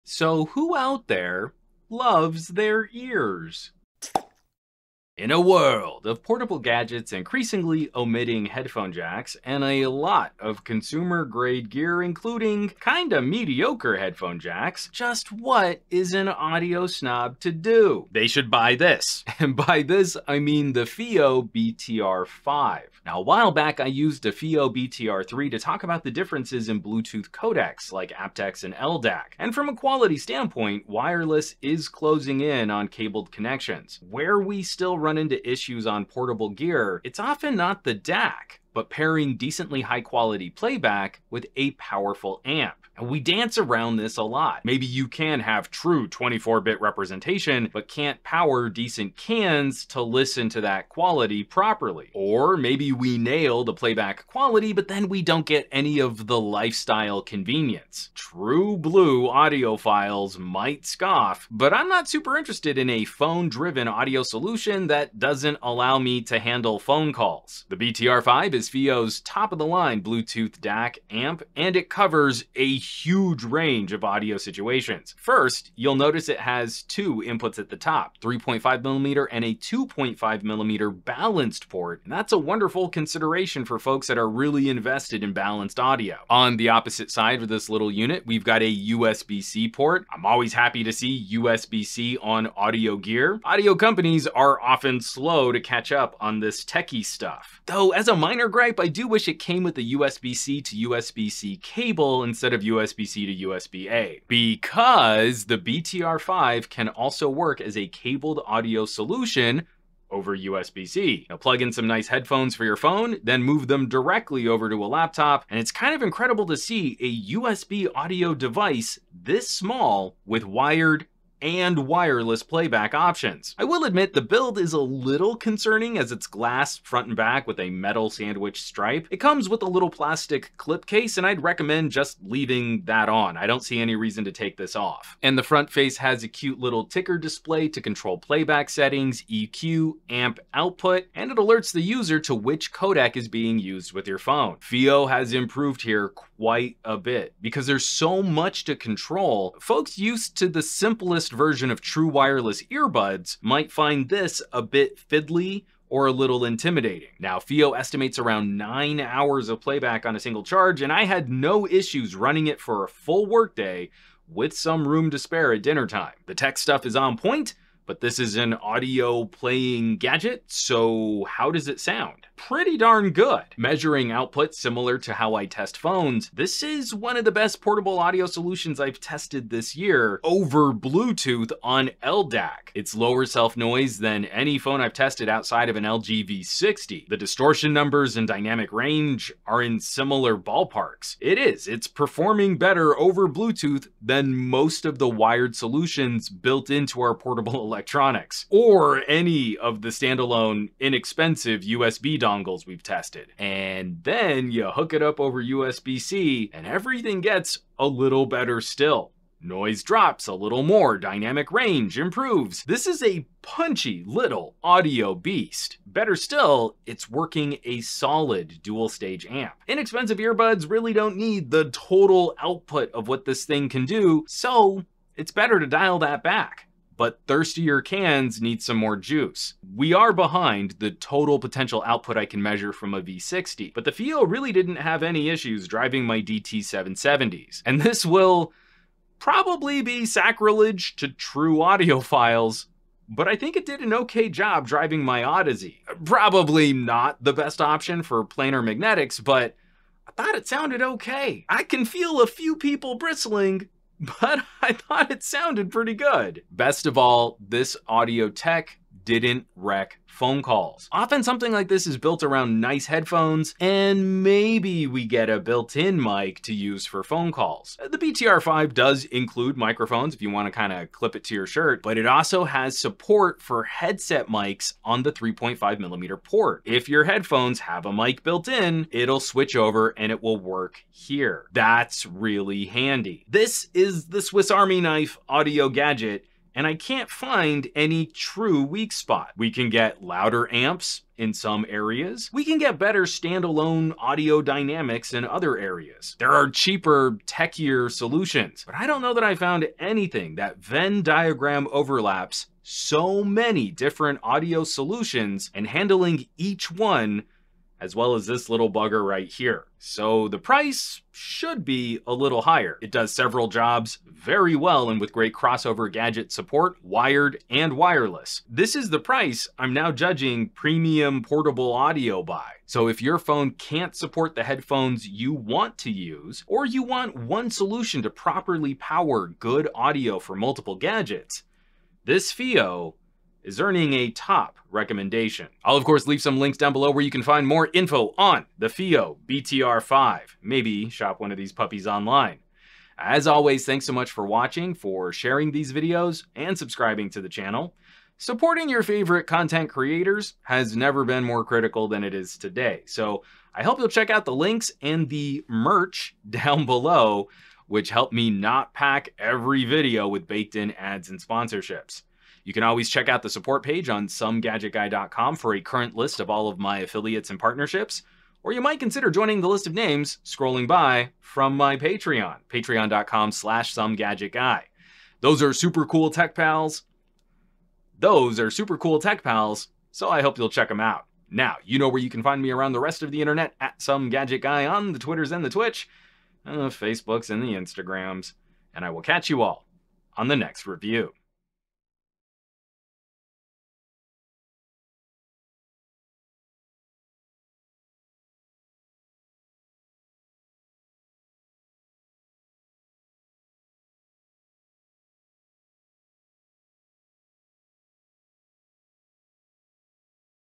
Fiio BTR 5 Bluetooth Headphone Amp: The CURE for a Missing Headphone Jack!
TREAT YOUR EARS! What are you supposed to do if your phone is missing the headphone jack? What do you do if your laptop has a mediocre headphone jack? Fiio has a Swiss Army Knife Bluetooth DAC that might be right up your alley! The BTR5 is a monster audio gadget. Let's take a look!
Fiio BTR 5 on Amazon